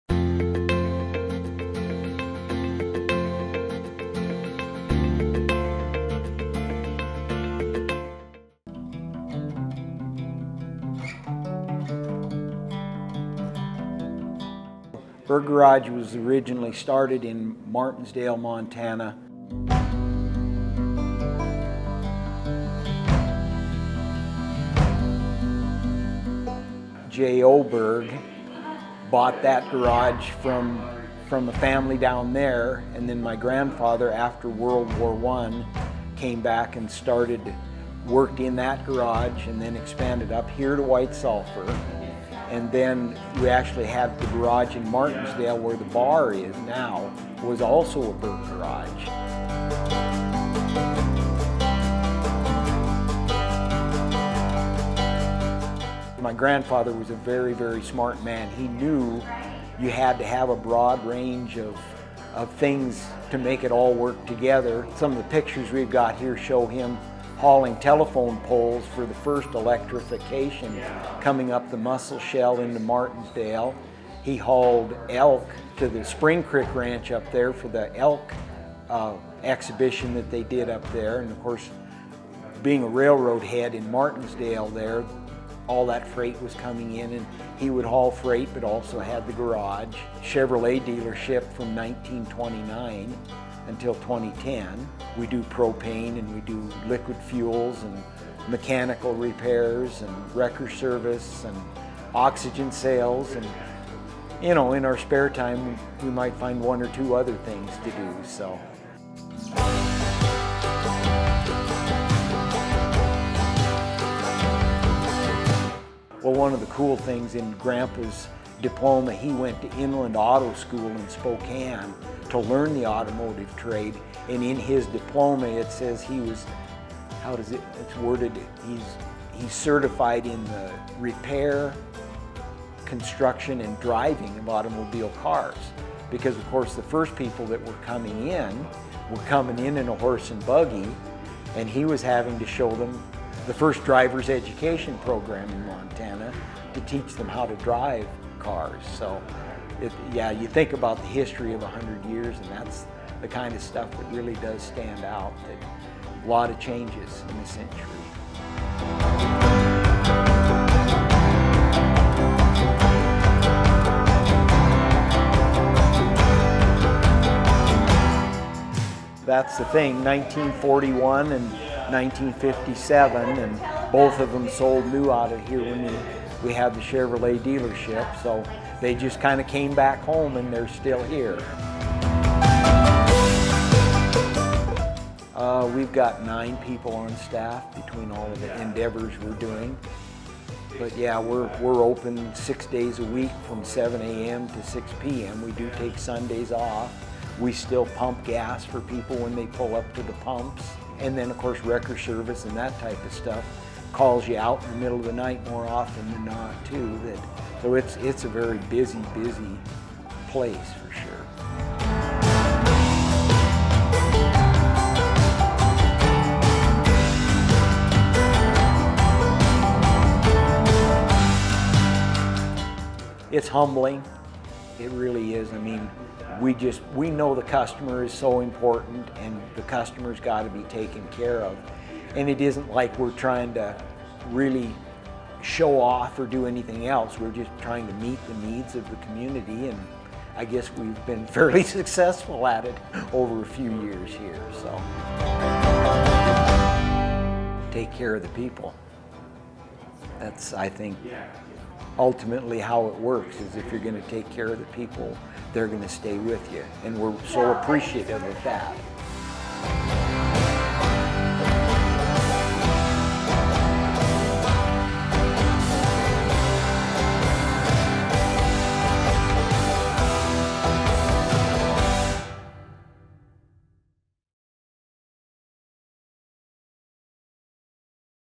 Montana Business Spotlight Berg Garage
Family-owned Berg Garage in White Sulphur Springs is featured in Montana Secretary of State Christi Jacobsen's "Montana Business Spotlight" series. Berg Garage has been in operation for more than 100 years, while first opening its doors in White Sulphur Springs in 1929.